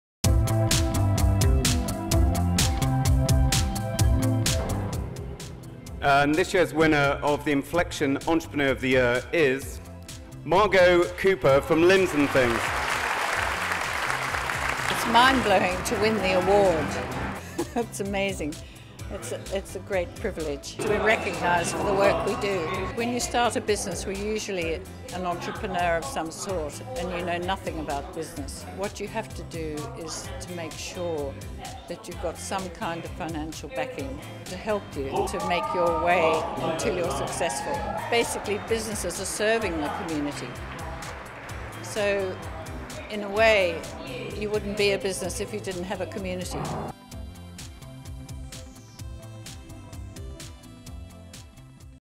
Margot Cooper receives The Inflexion Entrepreneur of the Year Award
At the Lloyds Bank National Business Awards, Margot Cooper won the prestigious The Inflexion Entrepreneur of the Year Award.

Cooper  is  the  founder  of  Bristol-based  Limbs  &  Things,  which  she  set  up  in  1990  after  having  a  vision  of  clinical  education  moving  away  from  cadavers  and  animals.  For  many  tasks,  these  were  difficult  to  use  and  expensive,  so  she  invented  anatomically  accurate  and  cost-effective  models  and  simulators  to  allow  students  -  whether  nurses,  GPs,  surgeons,  radiologists  or  paramedics  –  to  become  more  competent  and  confident  learners  and  professionals.